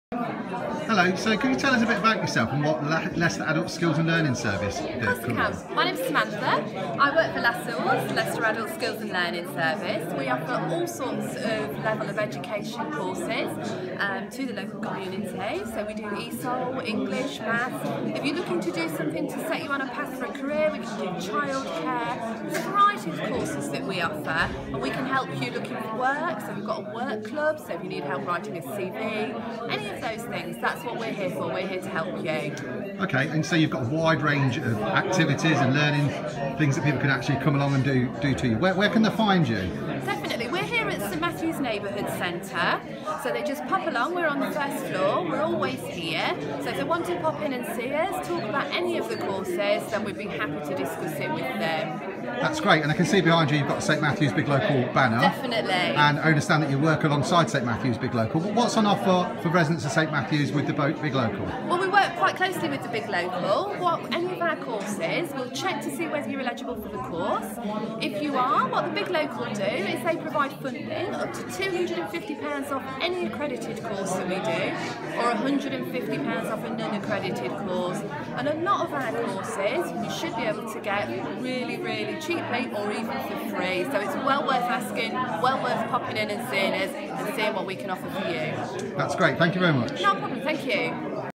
18 February 2016
St Matthews Big Local working with Leicester Adult Skills and Learning Service.

We are offering local St Matthews residents up to £250 off their courses and help with child care.